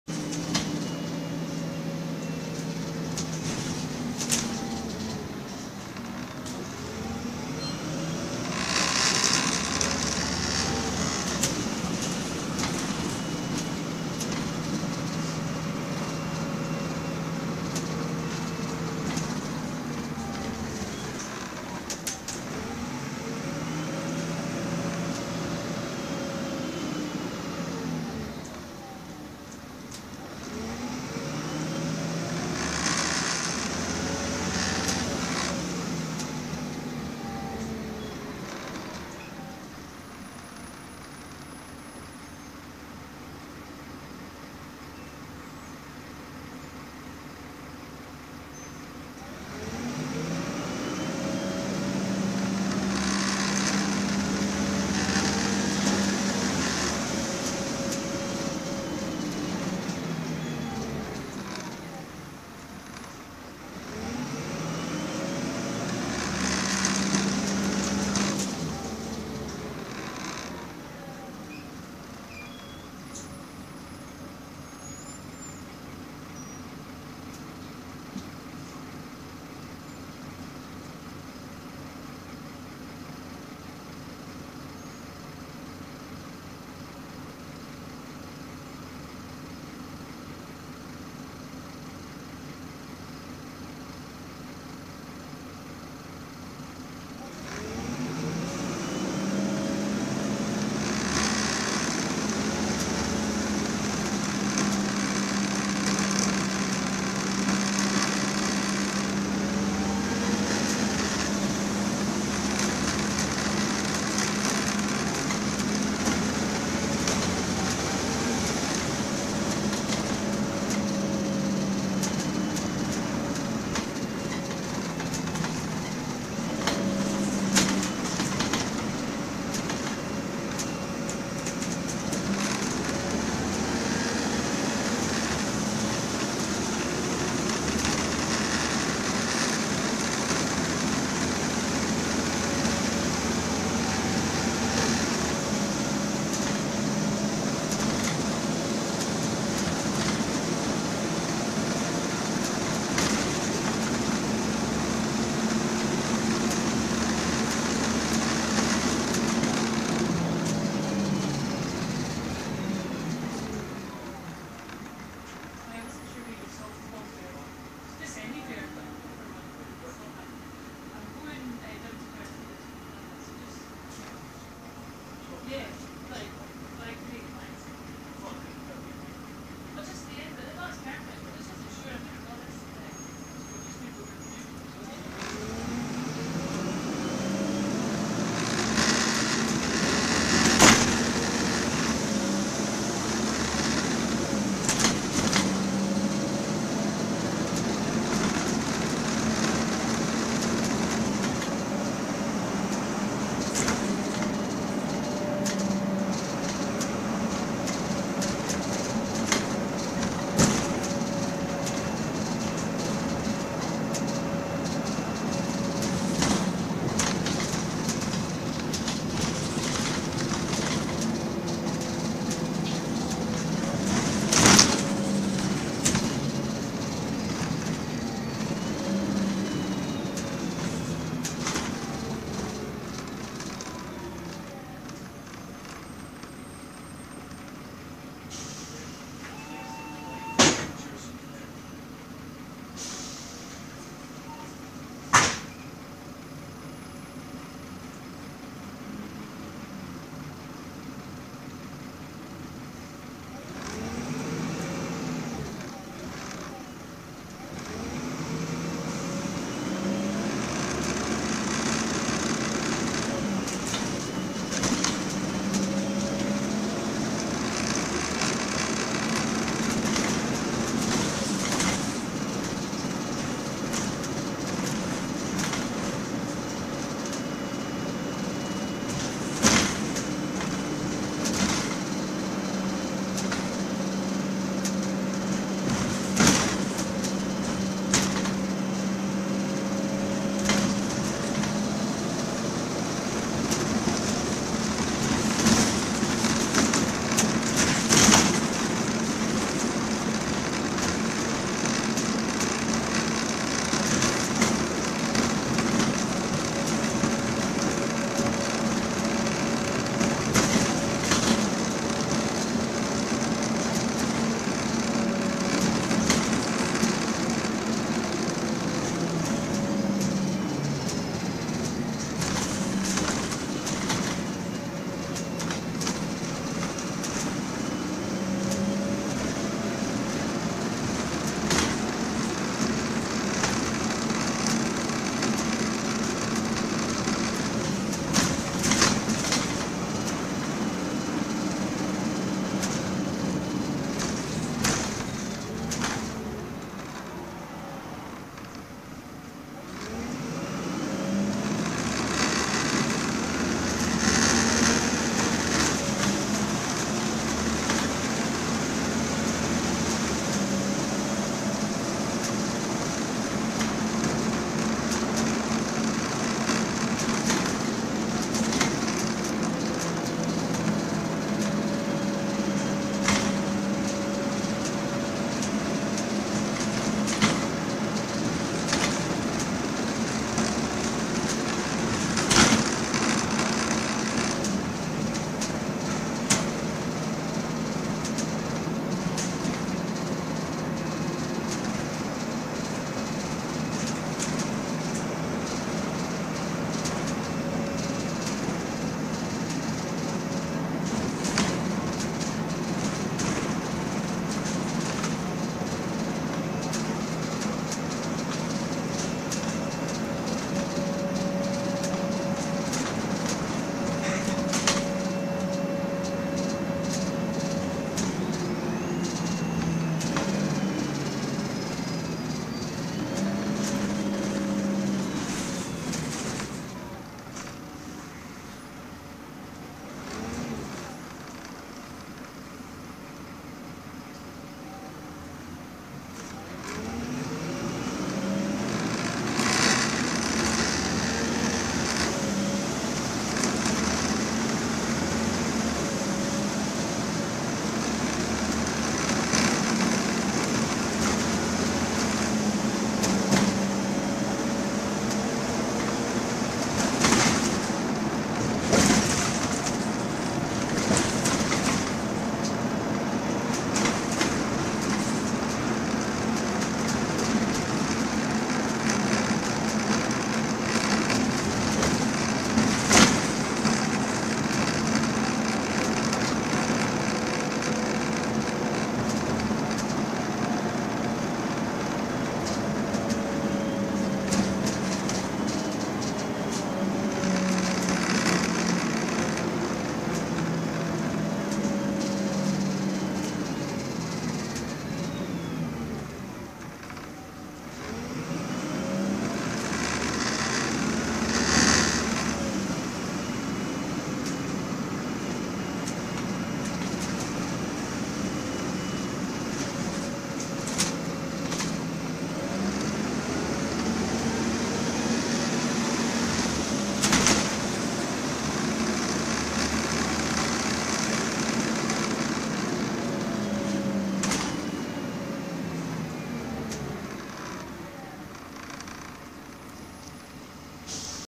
Stagecoach Strathtay MAN 18.240 Enviro 300 22568 SP08 DCO
22568 is seen here working the 1504 77 to Kingoodie.